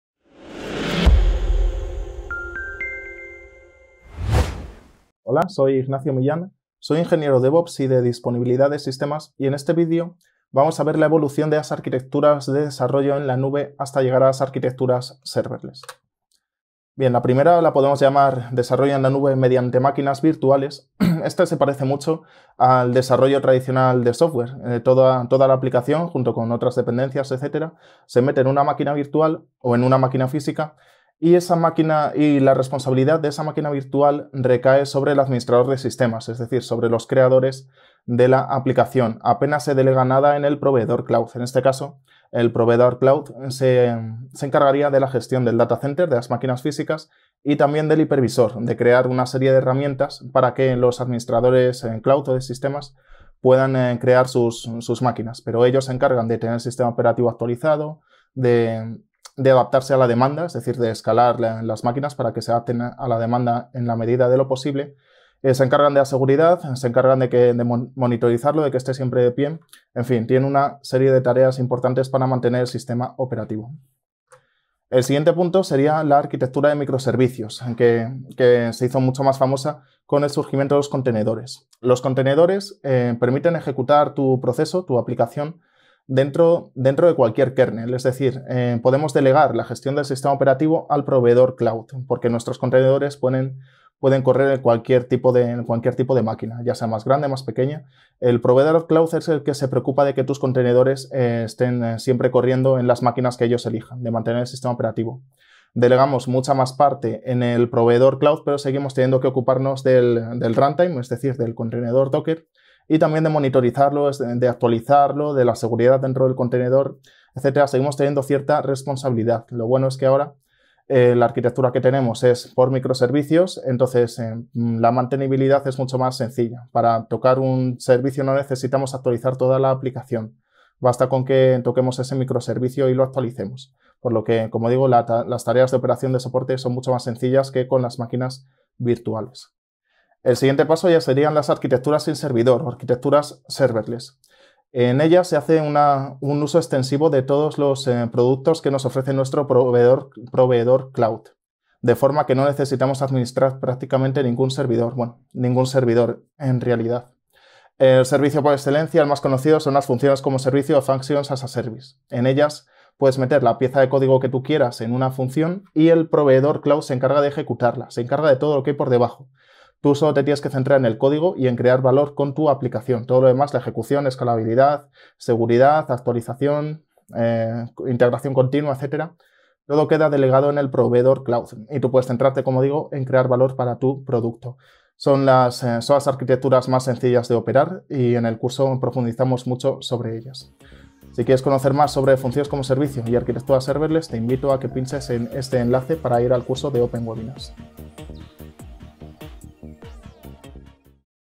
Evolución del Desarrollo de Software hasta llegar a Serverless
Descubre en este vídeo cómo ha sido la evolución de las arquitecturas de desarrollo de software hasta llegar a Serverless.

Índice:

1- Desarrollo en la nube mediante máquinas virtuales - 0:16
2- Desarrollo en la nube mediante microservicios - 1:14
3- Arquitecturas serverless - 2:19